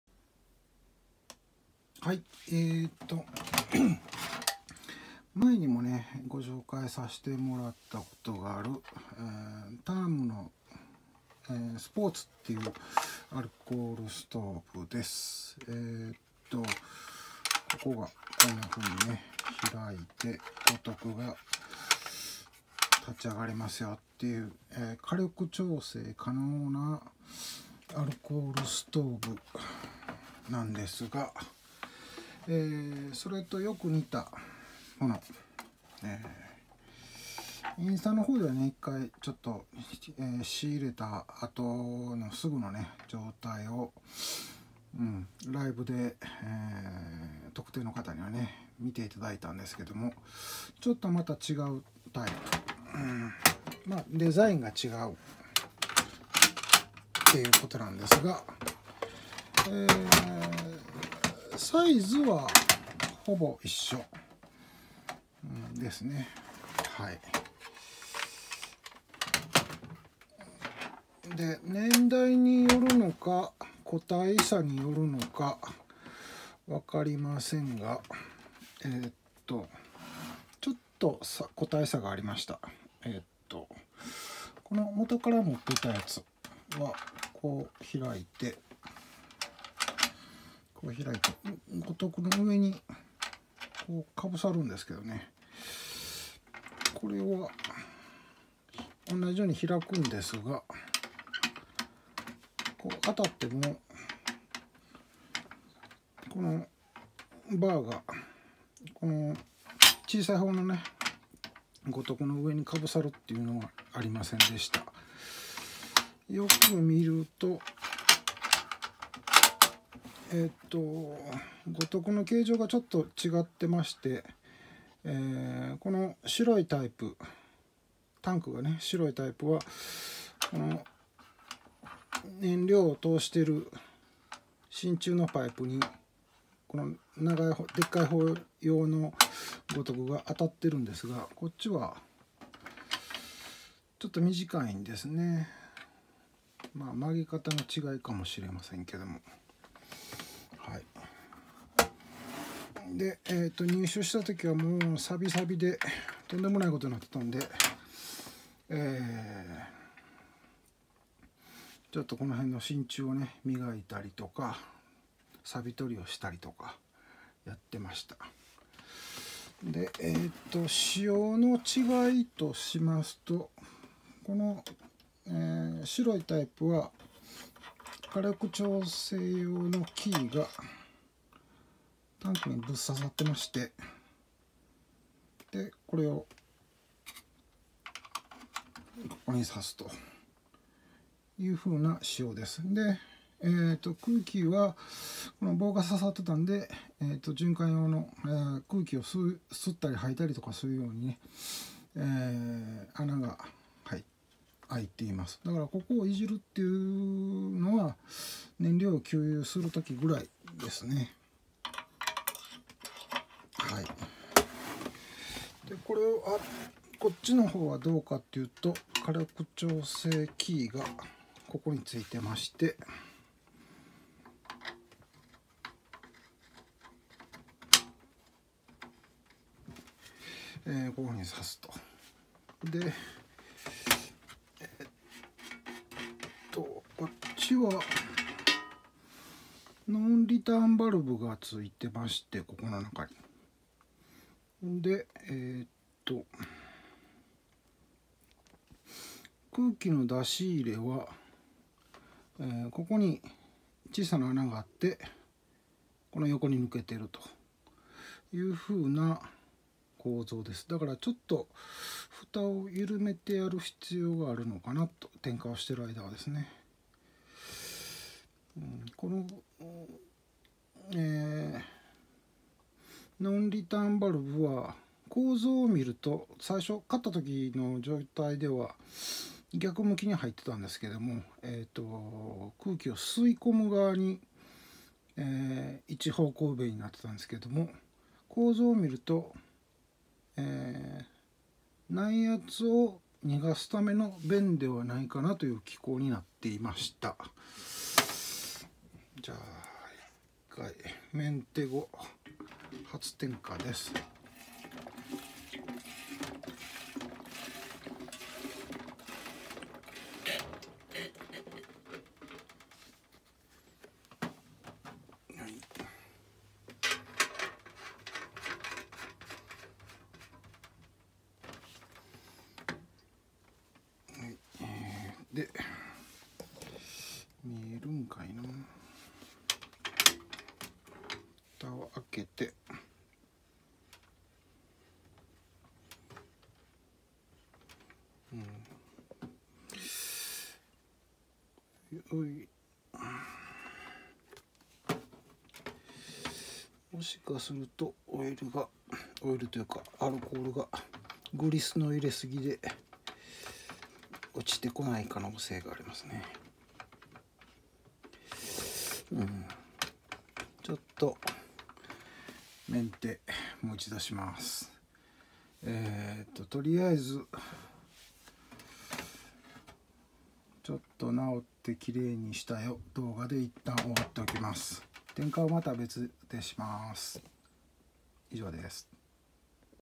TURMのアルコールストーブをまた手に入れた
ある動画でTURMのアルコールストーブをはじめて知TURMと言う火力調整可能なアルコールストーブを知ったきっかけの機種でした。

入手出来た物の余りの錆の多さにメンテして見ました。
ビフォー画像は無いです

組み立て間違いも有りこの動画では点火出来ずでした